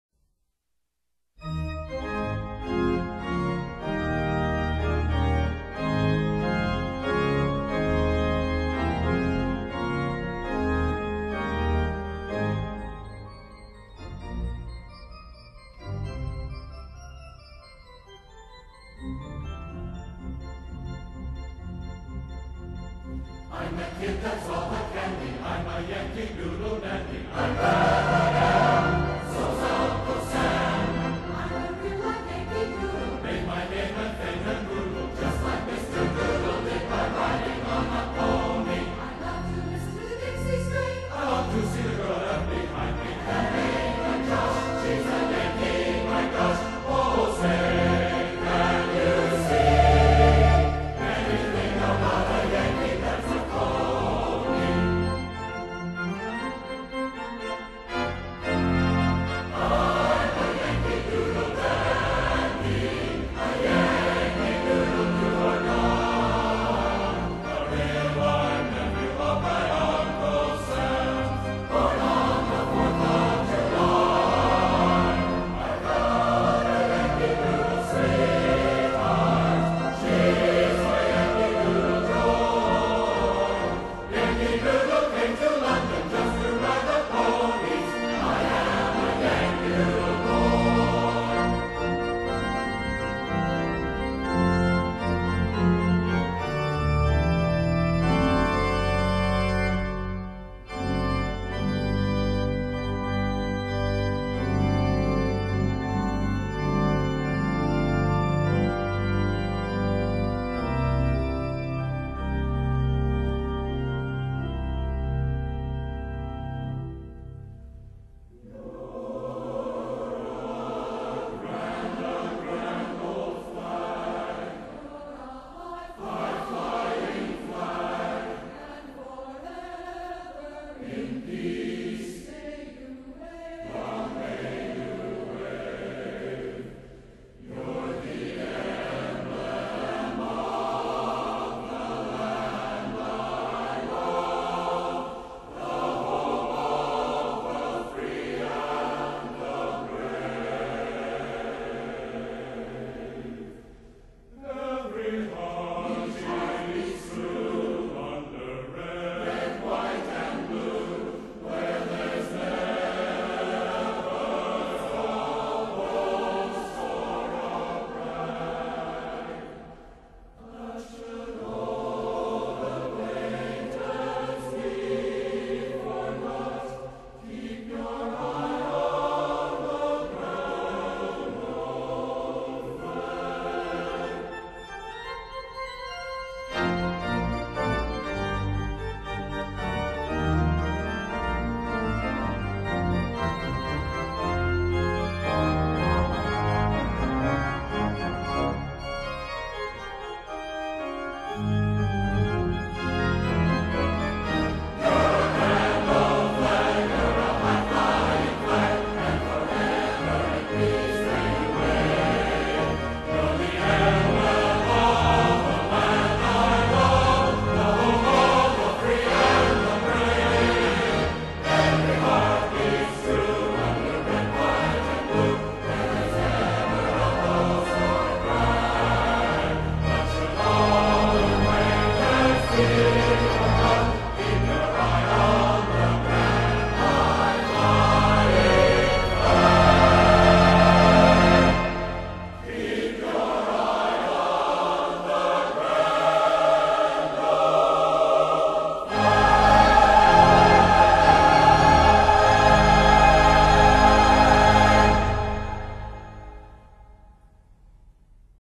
Weston Harris in Concert: Patriotic Medley
With the Southern California Mormon Choir,  Russell Fox director